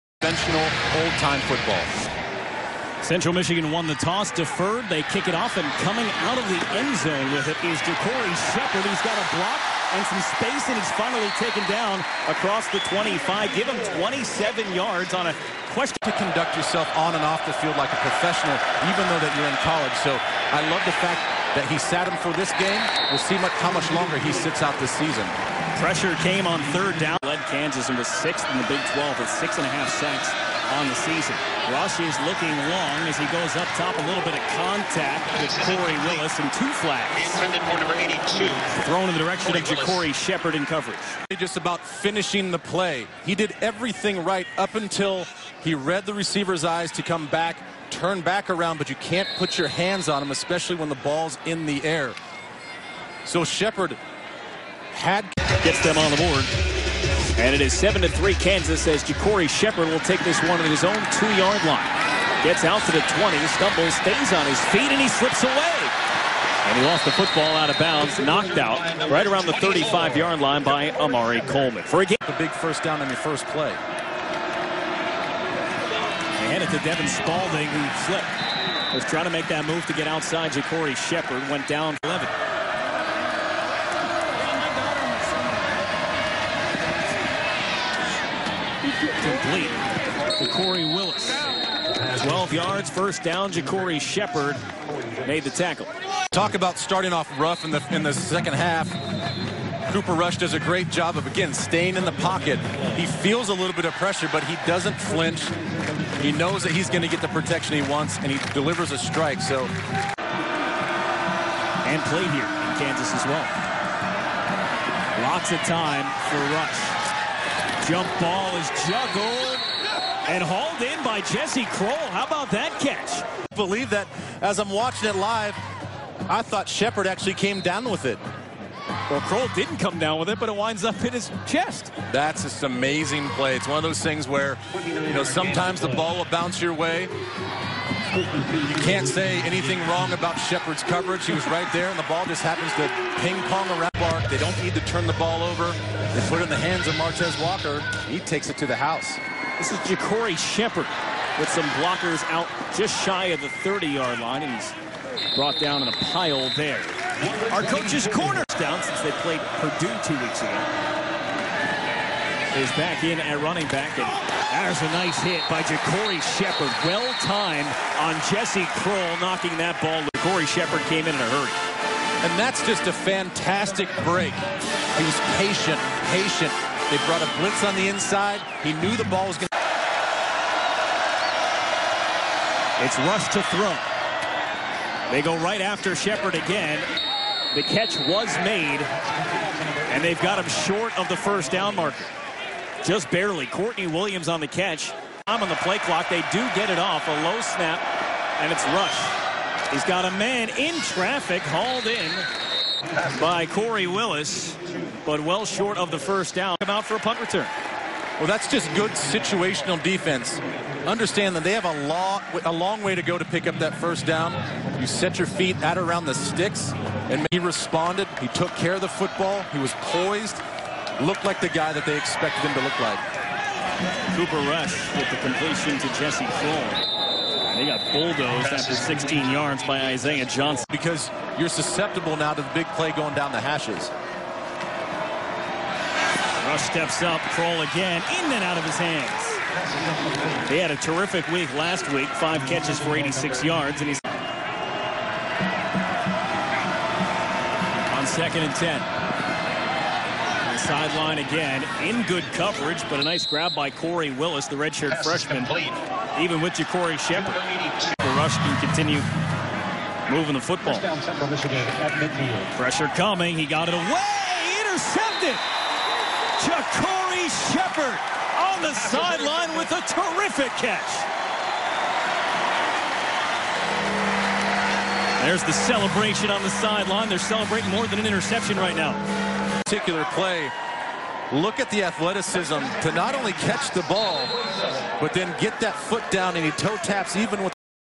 JaCorey Shepherd - Kansas Football - CB / KR - 2014 Central Michigan Game
JaCorey Shepherd Sr CB / KR KU vs. CMU 2014 Highlights
5'11" 190 lb 2014 All-Big12 1st Team 12 GP 30 Tkl 15 PBU 3 INT